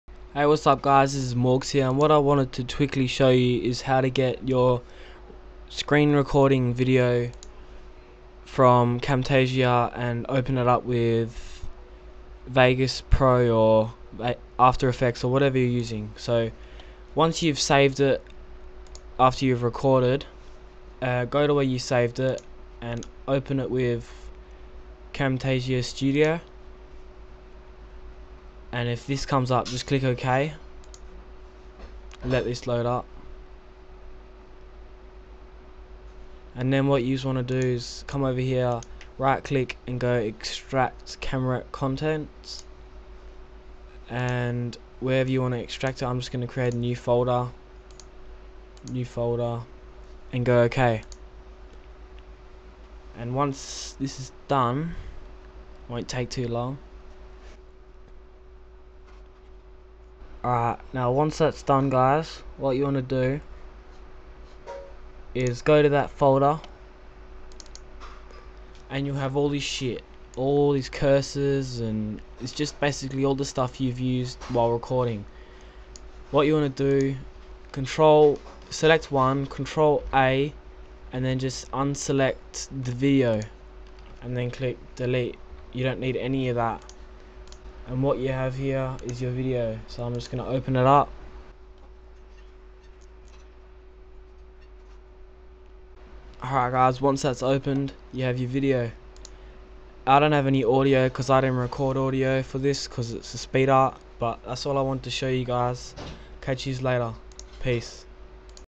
How To Use Camtasia Videos In Sony Vegas Pro
His Description:
"How to use Your Clips Or Video Recordings From Camtasia in Sony Vegas Pro or After Affects Basicly whatever your using you can put it into.

Please Tell Me if this was helpful?"

Send In Your Tutorials!